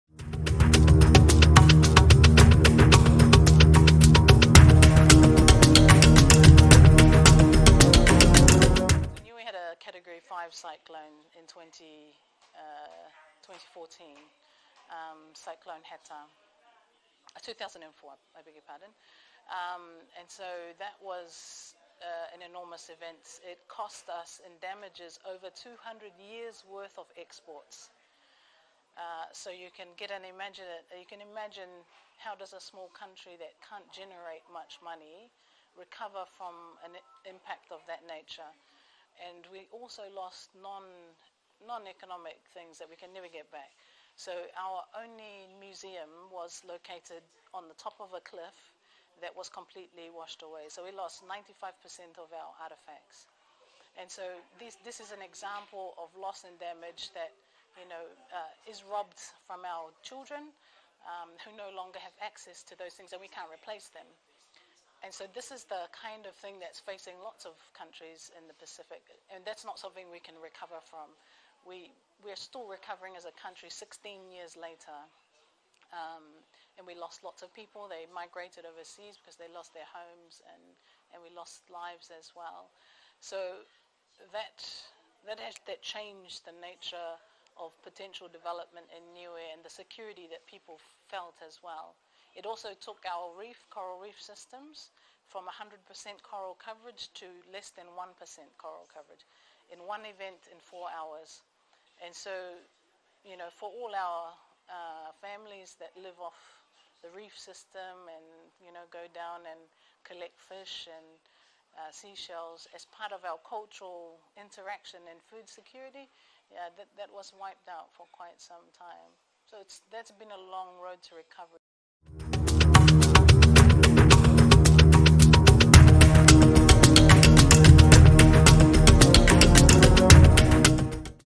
DAY 17 of 27 Days to COP27 27 days - Niue
Speaking at the recent Forum Economic Ministers Meeting in Vanuatu, Acting Director Climate Change & Environmental Sustainability, Pacific Community, Coral Pasisi, describes the devastations of the 2004 Cyclone Heta on the Rock of Polynesia - Niue.
Devastation so severe, the island nation is still recovering after 18 years.

The Pacific Island Forum Secretariat's '27 days to COP27' series is an information and awareness raising campaign supporting the Pacific OneCROP+ voice and momentum for Pacific priorities at the global climate negotiations.